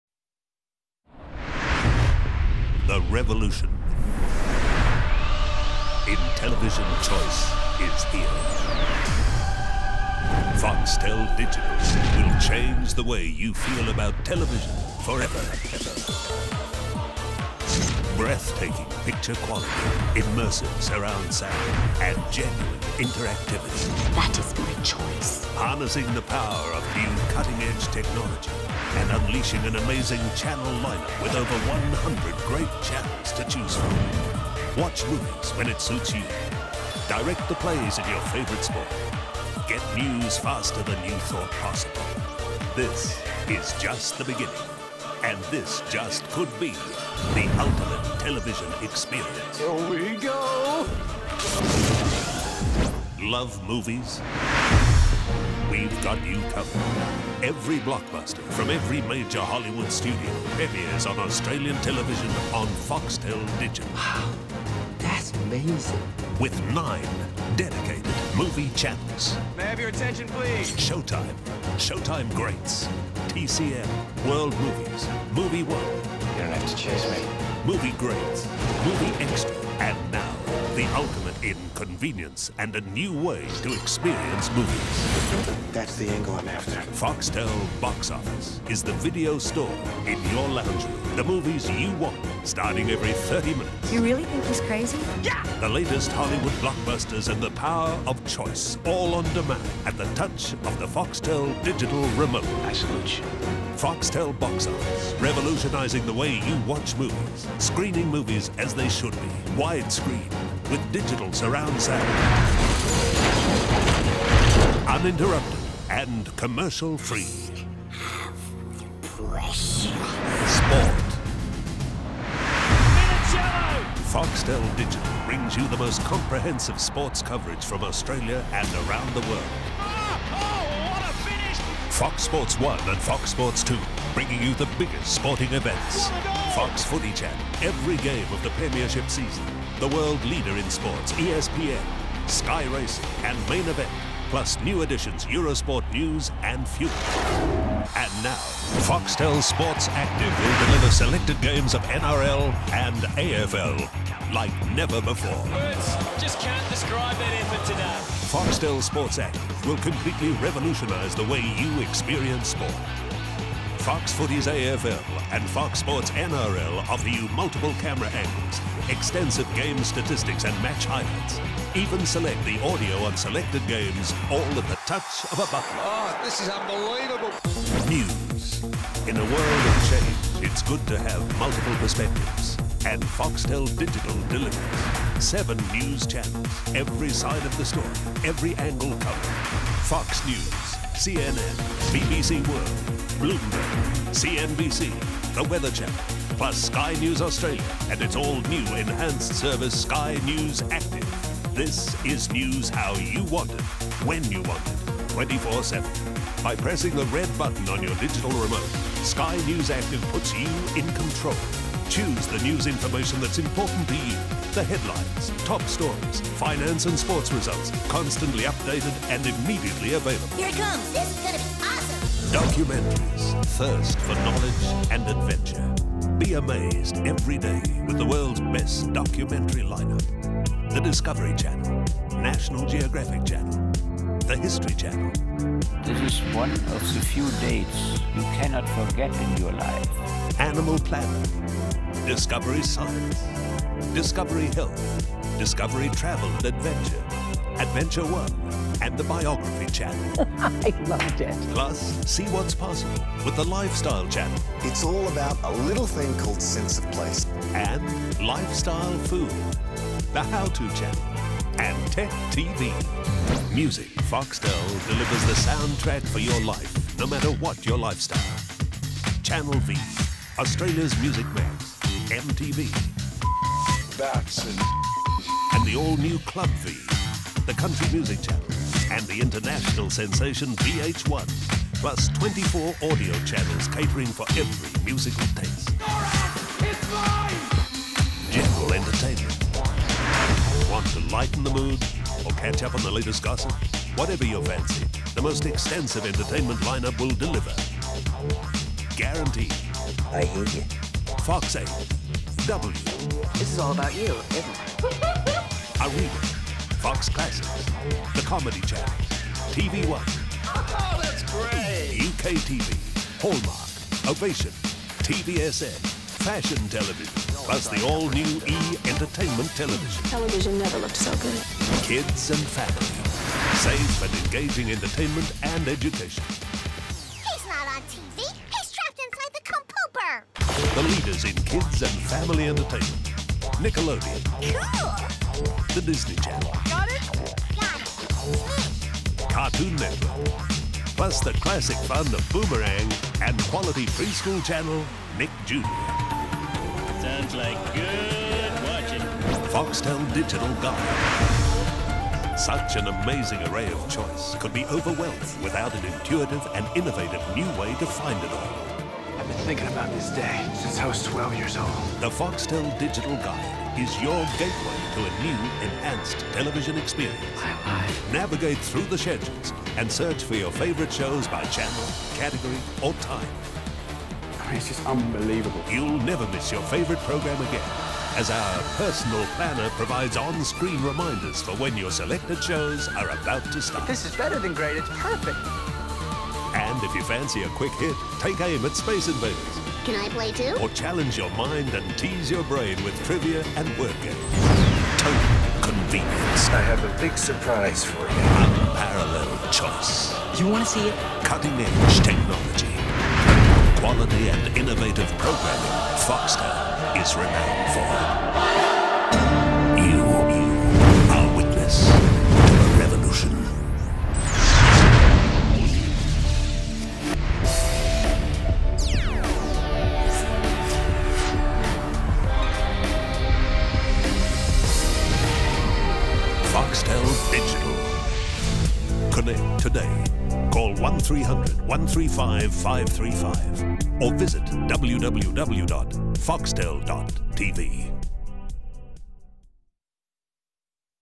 What is Foxtel Digital? - Foxtel Digital Australia 2004 Promo on DVD
property of foxtel. uploaded for historical references for what once was.